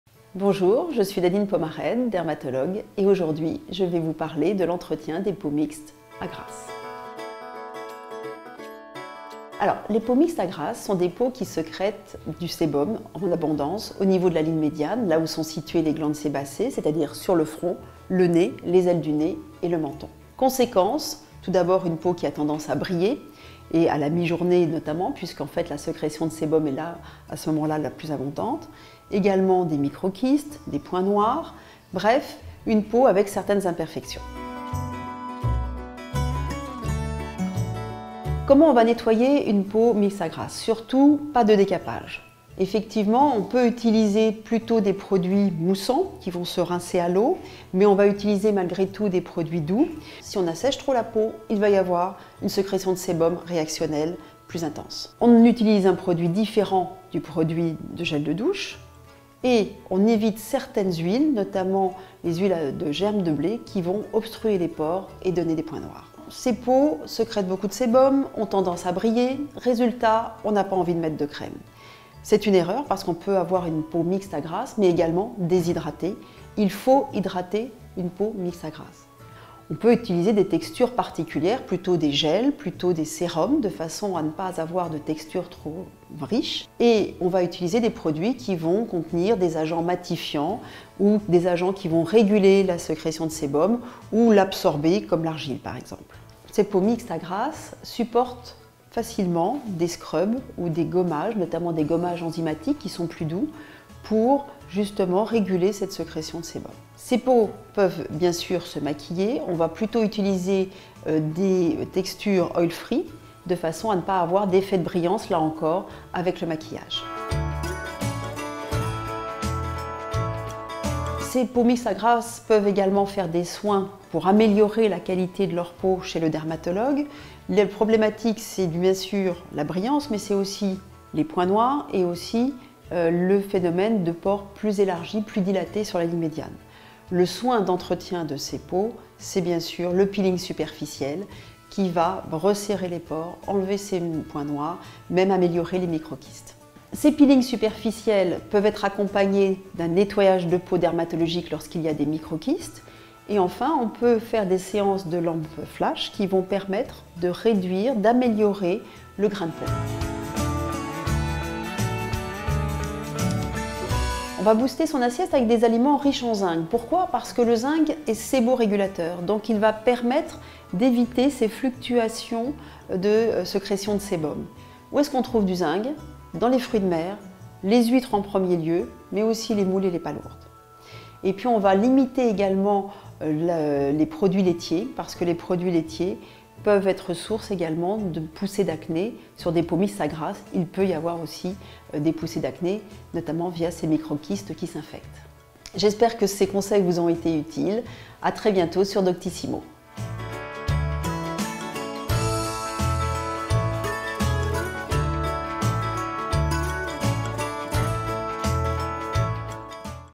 Comment prendre soin d'une peau grasse ?
Les peaux mixtes et grasses sont des peaux qui sécrètent du sébum en abondance sur le front, le nez, les ailes du nez et le menton. Conséquences : la peau brille, présente des microkystes, des points noirs… Comment éviter ces imperfections ? Comment soigner une peau grasse ? Y-a-t-il des produits à éviter ? Les conseils de Nadine Pomarède, dermatologue.

Comment prendre soin de la peau grasse ?

Production : LED